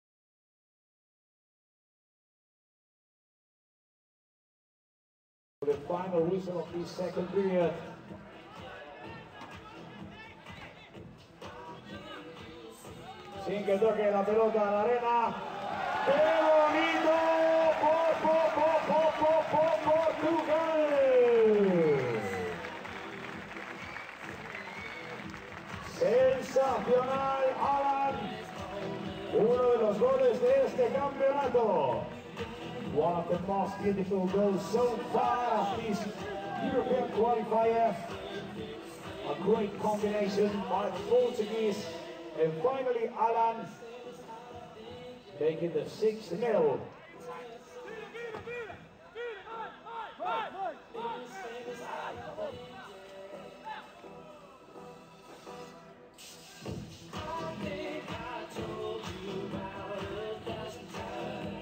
Por vs Est 6:0
FIFA Beachsoccer World Cup qualification in Spain, Castellon 2009. Portugese player Alan scores beautiful goal with his shoulder against Estonia.